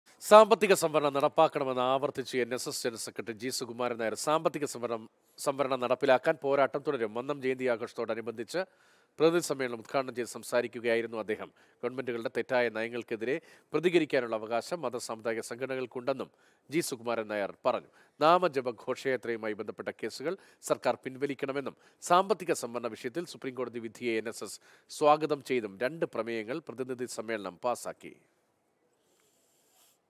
സാമ്പത്തിക സംവരണം നടപ്പിലാക്കണമെന്ന് ആവര്‍ത്തിച്ച് എന്‍.എസ്.എസ്. ജന.സെക്രട്ടറി
സാമ്പത്തിക സംവരണം നടപ്പിലാക്കണമെന്ന് ആവര്‍ത്തിച്ച് എന്‍.എസ്.എസ്. ജന.സെക്രട്ടറി ജി.സുകുമാരന്‍ നായര്‍

Happy viewing!